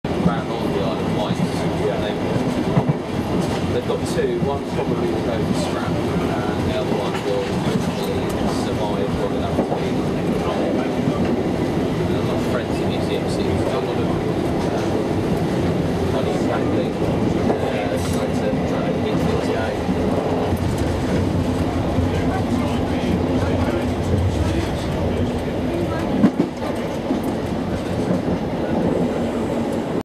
1938 Tube Stock To Carry Passengers
The London Transport Museum's historic 1938 tube stock train will be carrying passengers on Sunday 9th September 2018.